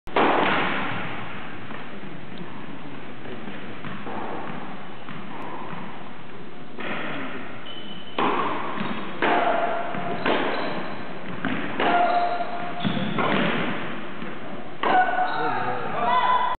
Agnieszka Radwanska in Luxembourg
Agnieszka Radwanska playing a point against Victoria Azarenka in Luxembourg, 2007